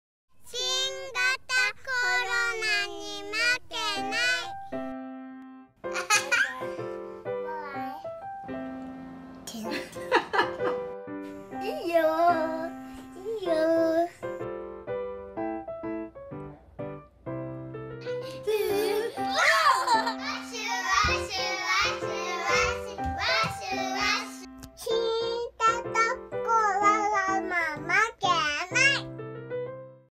新型コロナに負けない スマイル編その8
新型コロナに負けない動画募集第2弾スタート。
みんなが笑顔になれる可愛い動画を送ってください。
投稿はRBCアプリからお待ちしています。
詳しくは「RBC　新型コロナに負けない」で検索。